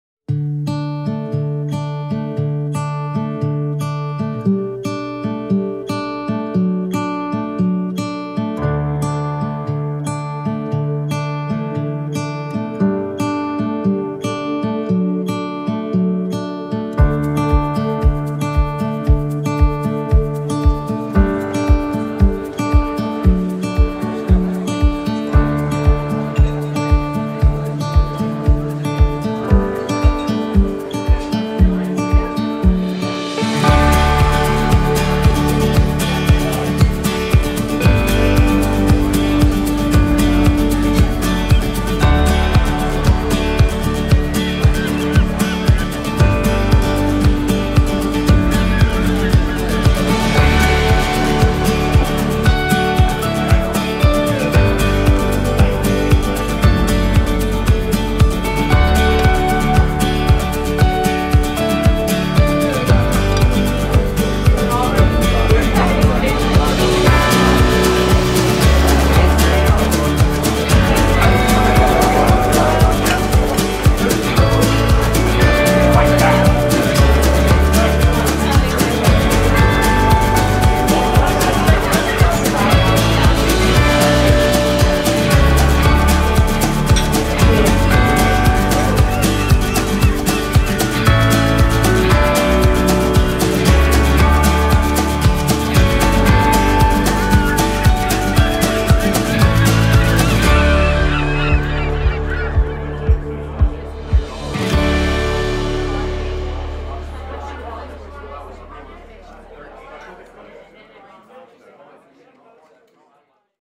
Tuman Cafe, Keelung, Taiwan
Keelung is located in the northeast of Taipei. One day when I went to explore their coastal area we found a beautiful, cozy cafe that is facing the water. Great foodie spot to check out if you ever find yourself visiting Taiwan.